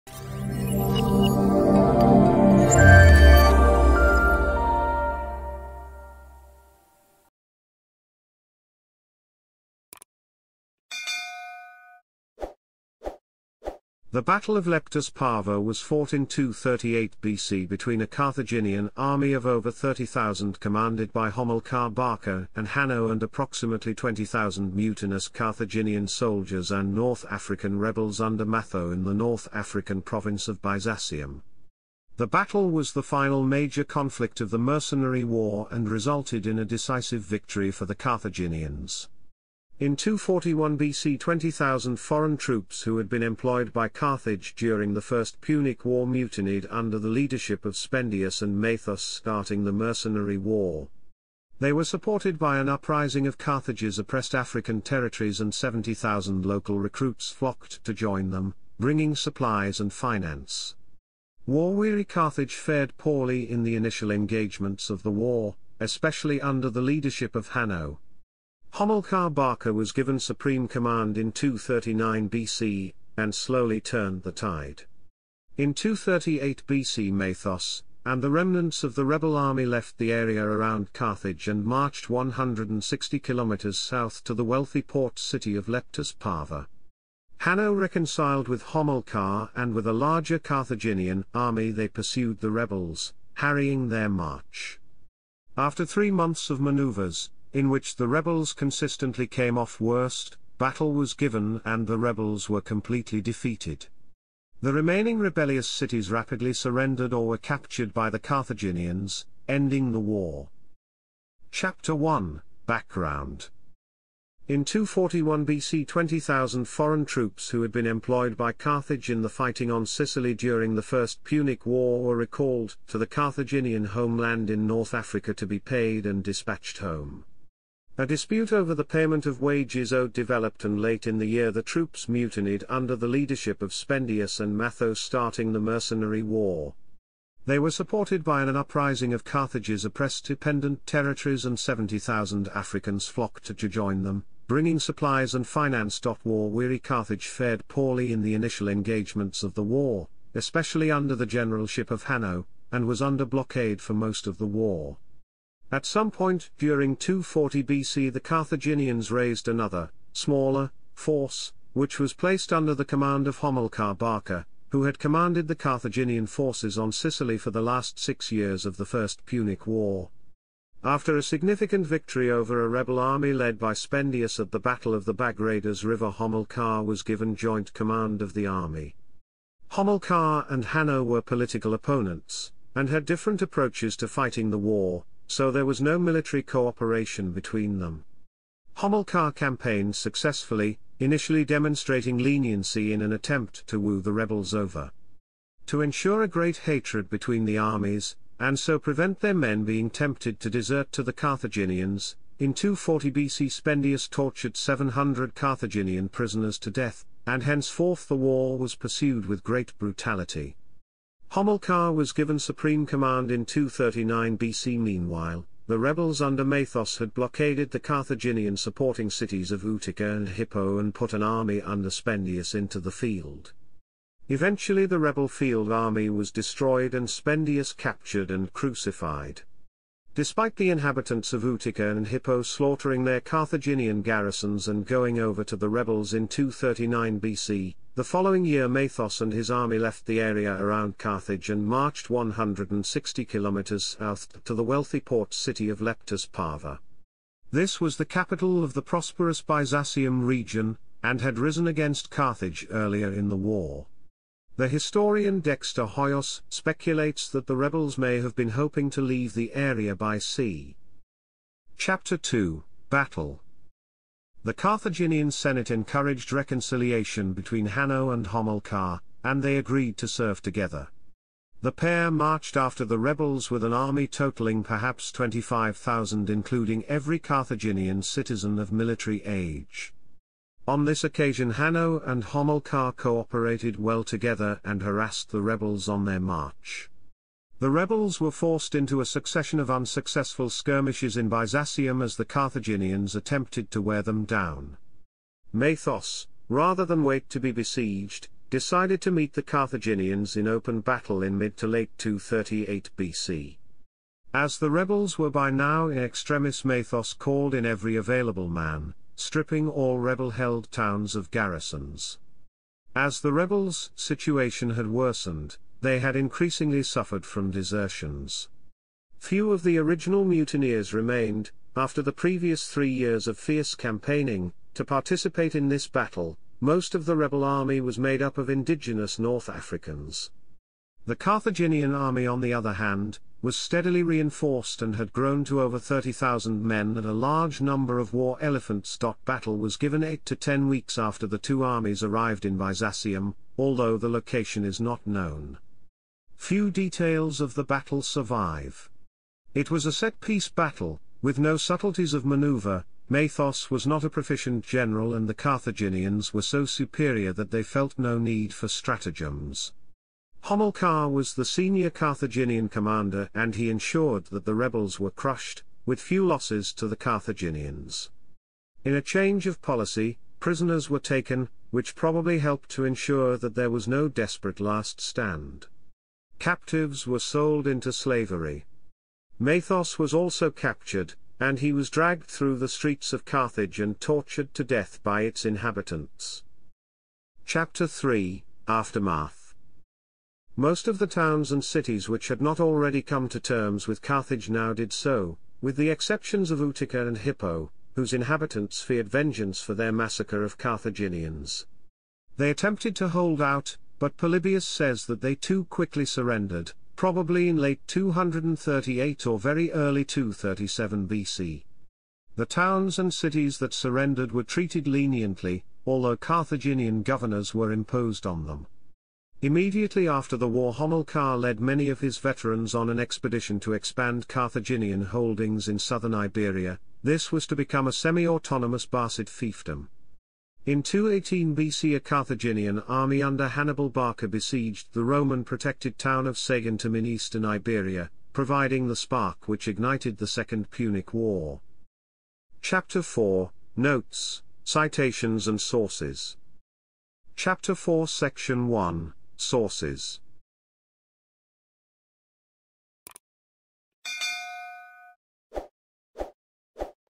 Battle of Leptis Parva - Wikipedia article
Video summary:
The Battle of Leptis Parva was fought in 238 BC between a Carthaginian army of over 30,000 commanded by Hamilcar Barca and Hanno and approximately 20,000 mutinous Carthaginian soldiers and North African rebels under Matho in the North African province of Byzacium (in modern Tunisia). The battle was the final major conflict of the Mercenary War and resulted in a decisive victory for the Carthaginians.